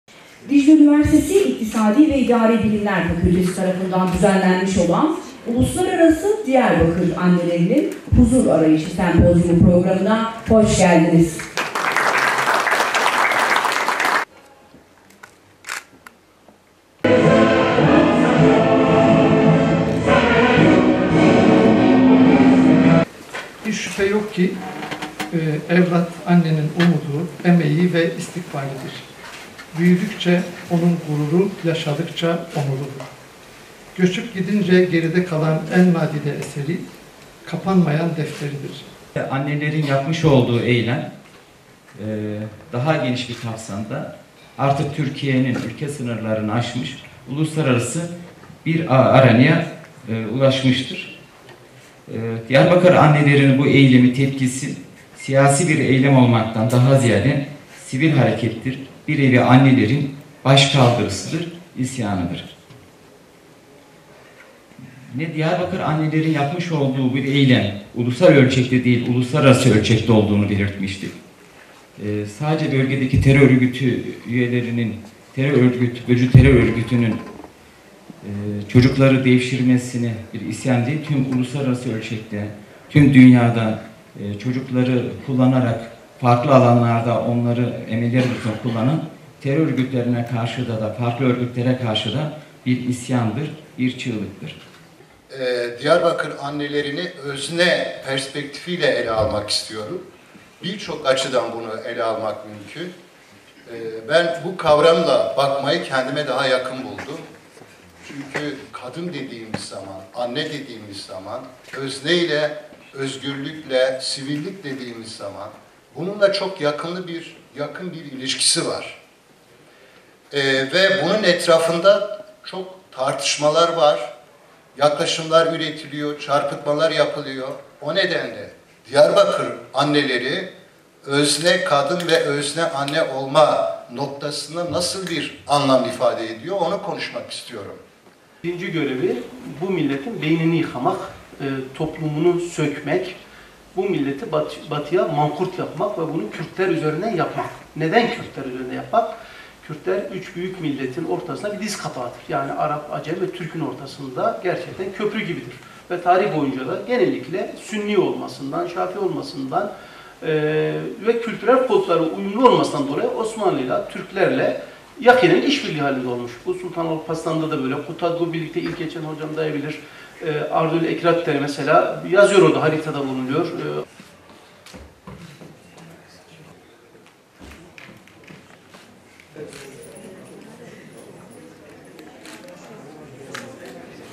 “ULUSLARARASI DİYARBAKIR ANNELERİNİN HUZUR ARAYIŞI” SEMPOZYUMU ÜNİVERSİTEMİZ EV SAHİPLİĞİNDE BAŞLADI
Dicle Üniversitesi İktisadi ve İdari Bilimler Fakültesi tarafından düzenlenen “Uluslararası Diyarbakır Annelerinin Huzur Arayışı” sempozyumu başladı.

15 Temmuz Kültür ve Kongre Merkezinde gerçekleşen sempozyumun açılış programına Rektörümüz Prof. Dr. Mehmet Karakoç, MKYK Üyesi Doç. Dr. Ali Karakaş, Rektör Yardımcıları, STK Temsilcileri ve Kamu-Kurum Temsilcileri, Dekanlar, Akademisyenler ve öğrenciler katılım sağladı.